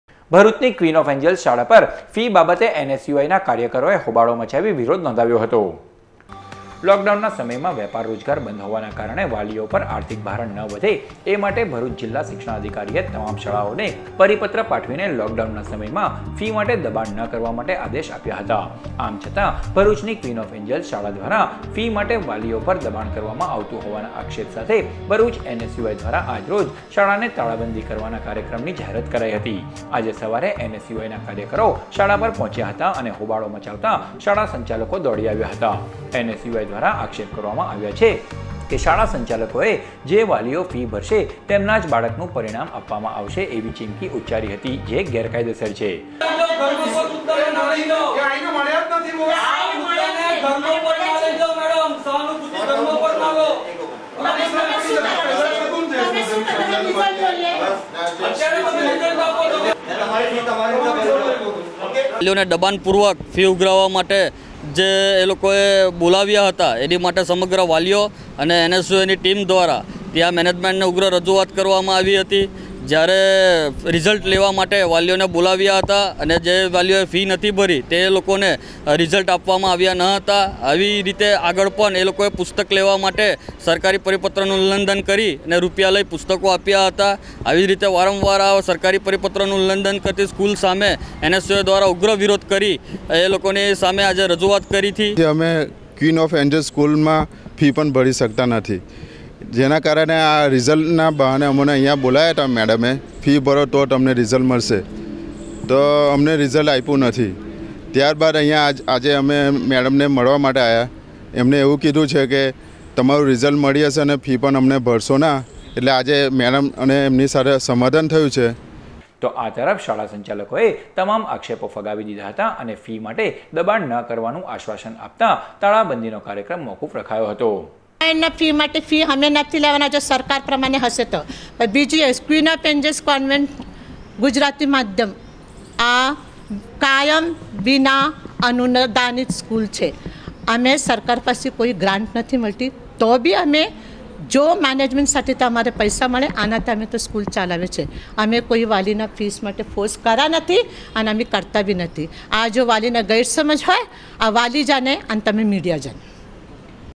narmada samachar
ભરૂચની ક્વીન ઓફ એન્જલ શાળા પર ફી બાબતે એન.એસ.યુ.આઈ.નાં કાર્યકરોનો હોબાળો... લોકડાઉનના સમયમાં પણ વાલીઓ પર ફી બાબતે દબાણ કરાતું હોવાના આક્ષેપ... પ્રશ્નના નિરાકરણની ખાતરી મળતા શાળાને તાળાબંધીનો કાર્યક્રમ મોકૂફ રાખ્યો